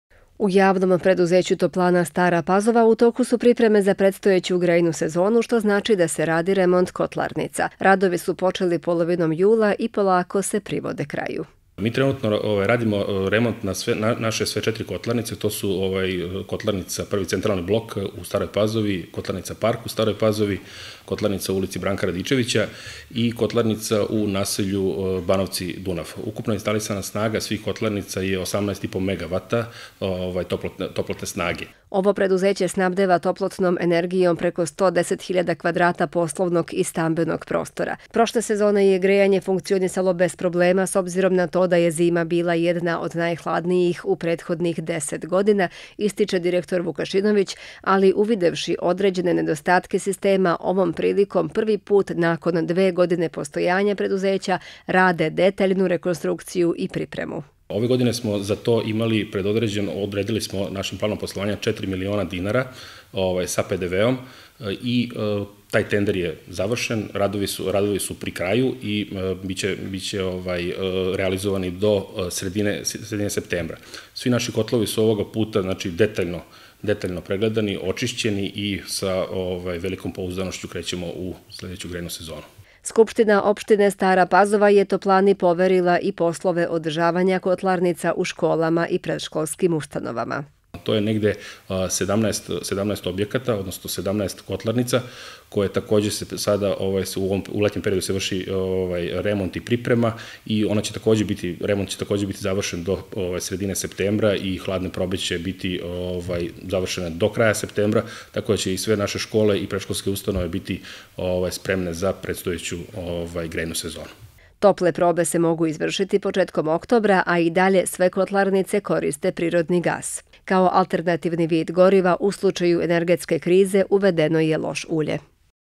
Remont kotlarnica
Letnji period donosi JP Toplana Stara Pazova  najviše posla, jer se tad vrše poslovi oko remonta svih kotlarnica i toplotnih podstanica.  Ove godine se rade detaljni pregledi i čišćenja, koji su počeli polovinom jula, i do polovine septebra bi trebalo da budu završeni.